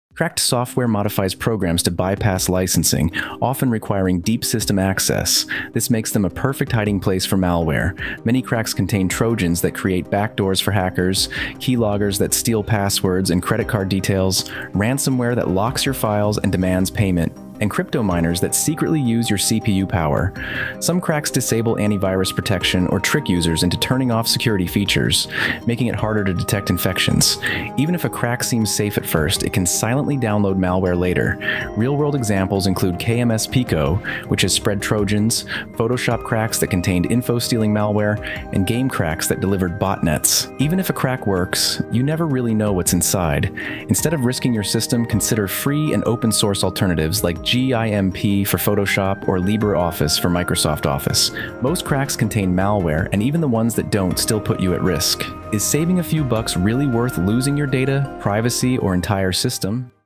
Do cracks contain virus? (in 1 min)
Welcome to: Do cracks contain virus? (in 1 min)
In this short video, we explain whether cracked software is safe or dangerous, how cracks may contain malware, and why using pirated software can harm your PC. 💻⚠️